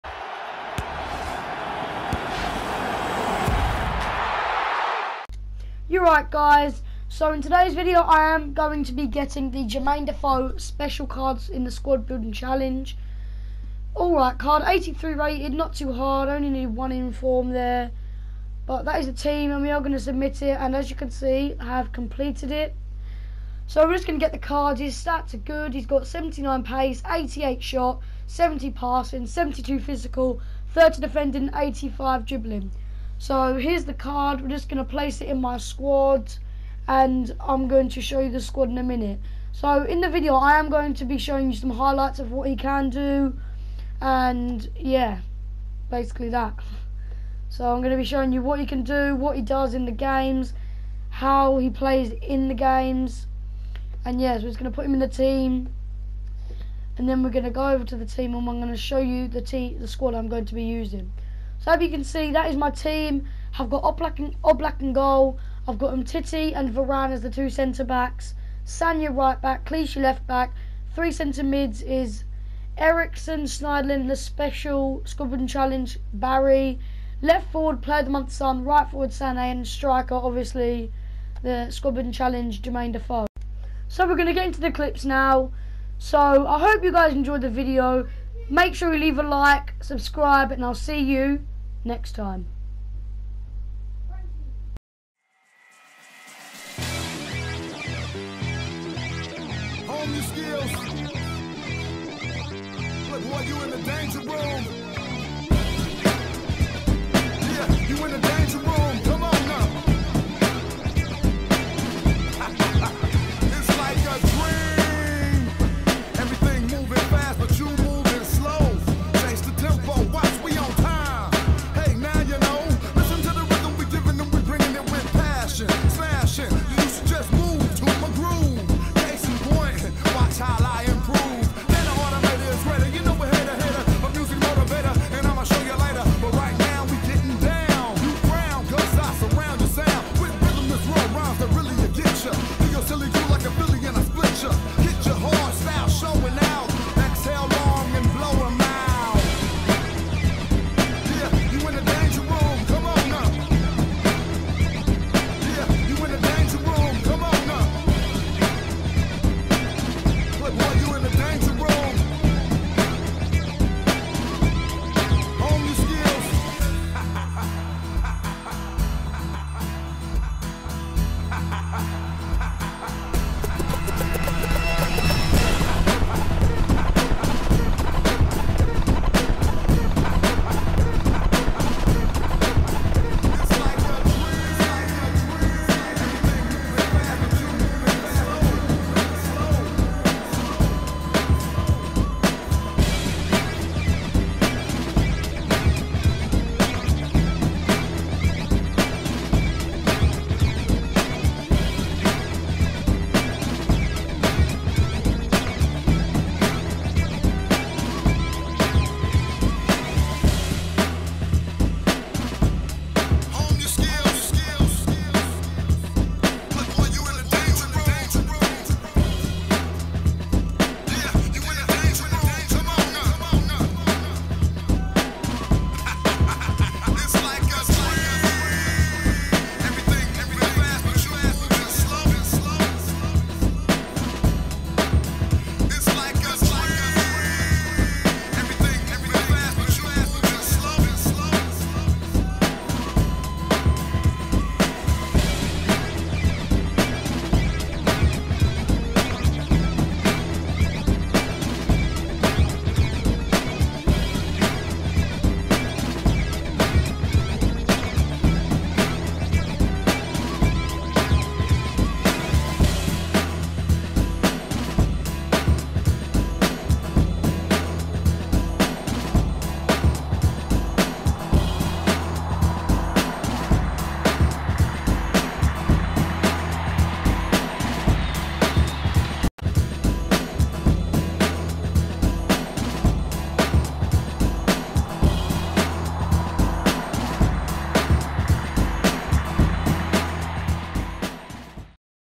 SBC Jermain Defoe!!!
In this video I used my SBC Jermain Defoe and showed you some of his highlights when I was playing with him.